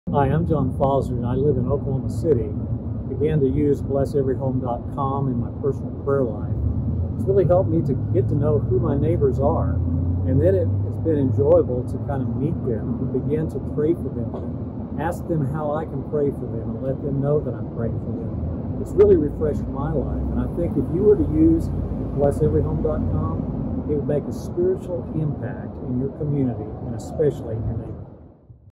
You Can Share: John Fozard
John Fozard, President, Mid-America University, tells how he uses Bless Every Home.